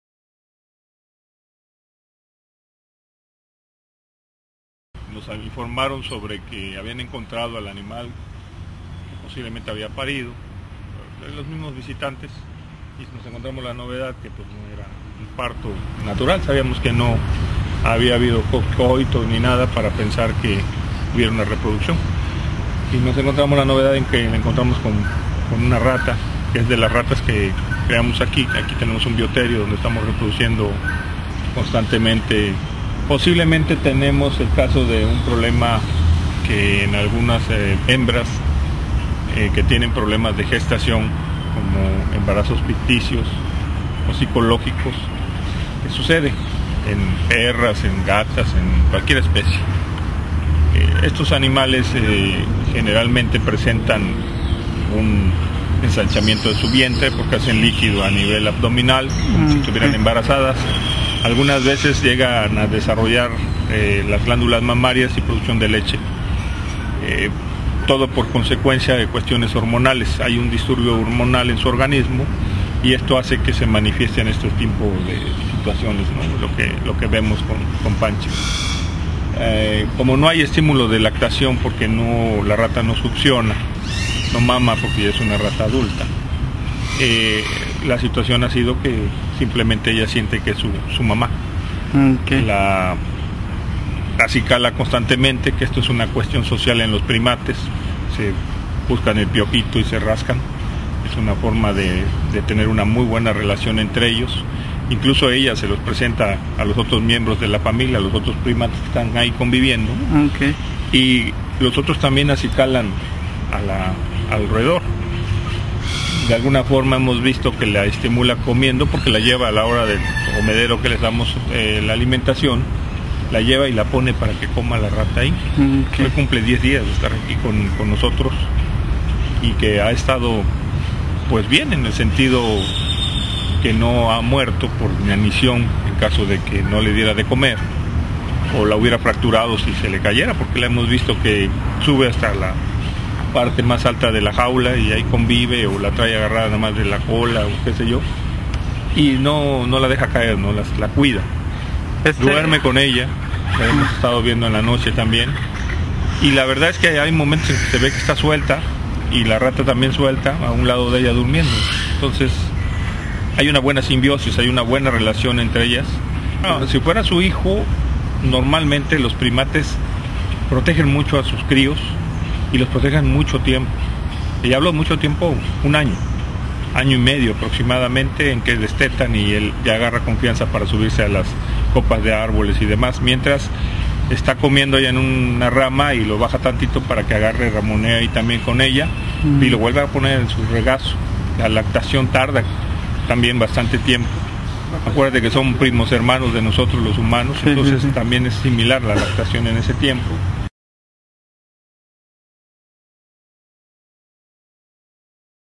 Ratanga dijo la Changa
Récord histórico de asistencia en parque Viveros por "Pancha" y la rata. Debido al auge que ha generado la historia del mono araña que adoptó una rata siria.